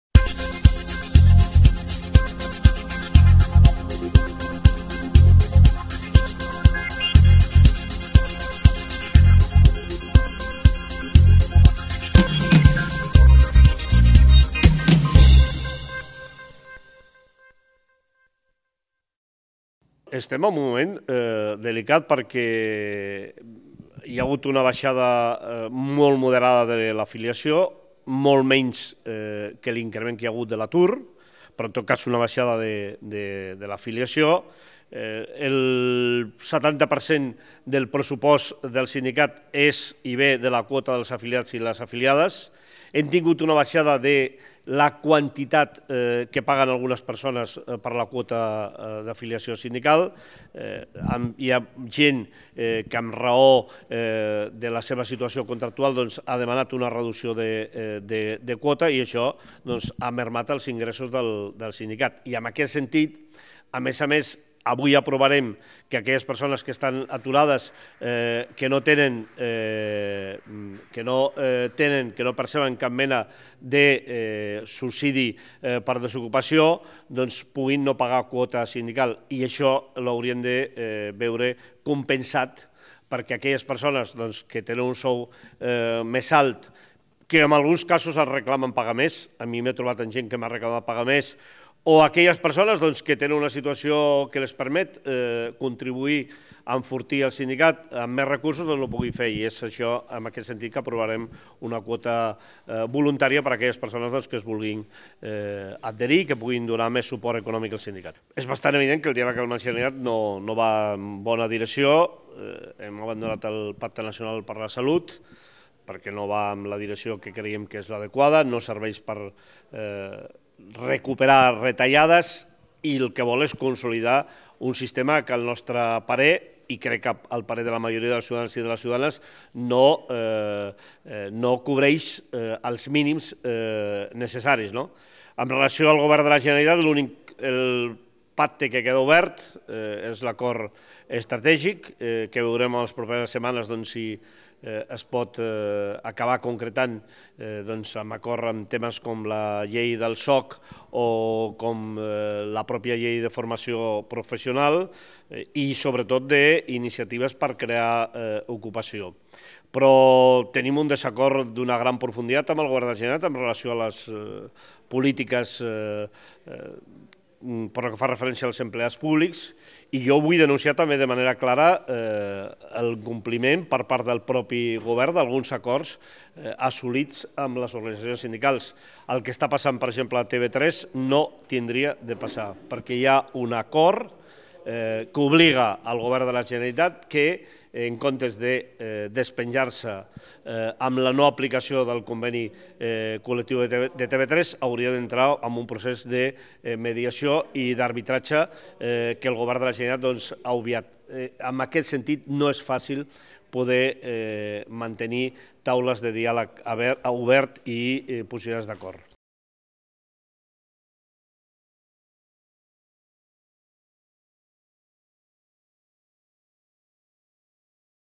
La UGT de Catalunya aprova una 'quota 0' per a aturats i una 'quota suport' voluntària
El Comitè Nacional de la UGT de Catalunya ha aprovat l'aplicació d'una "quota 0" per a aquells afiliats aturats que no tenen cap ingrés, així com una "quota suport" (la quota d'afiliació ordinàira s'augmentaria fins als 15 euros mensuals) per a aquells afiliats que puguin i vulguin augmentar la seva contribució al sindicat, en un moment econòmic complicat. Ho explica al vídeo el secretari general de la UGT de Catalunya, Josep M. Àlvarez, qui també denuncia que el diàleg amb la Generalitat no va bé. Un exemple és que la UGT de Catalunya ha abandonat el Pacte Nacional per la Salut.